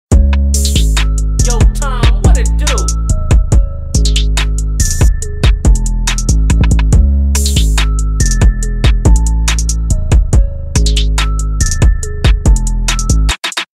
Sur Ron Wrap
Sur Ron Graphics, Surron wrap kit for the electric dirtbike.
I helped my friends install a graphics kit on his Sur Ron X Electric Dirt Bike